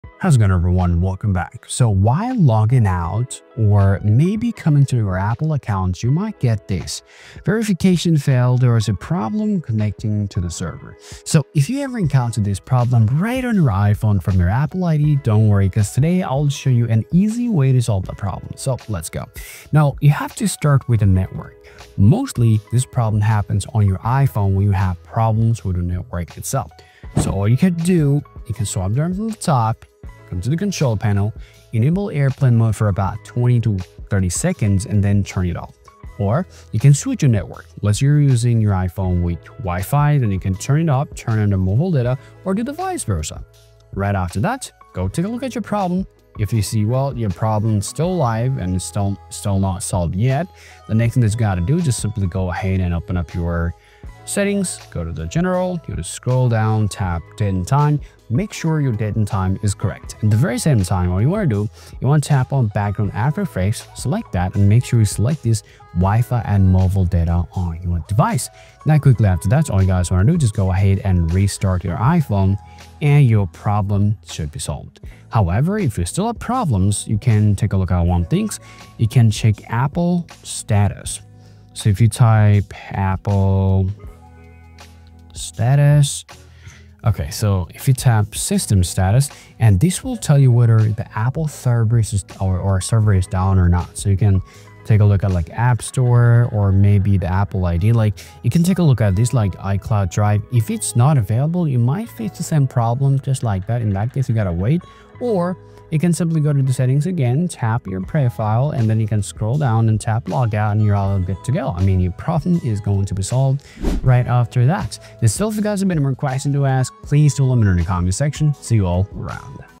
Fix Verification Failed: There Was a Problem Connecting to the Server on Apple ID on iPhone
Facing the "Verification Failed: There Was a Problem Connecting to the Server" error on your Apple ID? Don’t worry! In this video, I'll guide you through simple yet effective solutions to fix this issue on your iPhone. Whether it’s a connection problem, incorrect settings, or server-side issues, we’ll cover all the troubleshooting steps to help you resolve the verification error and get your Apple ID working smoothly again.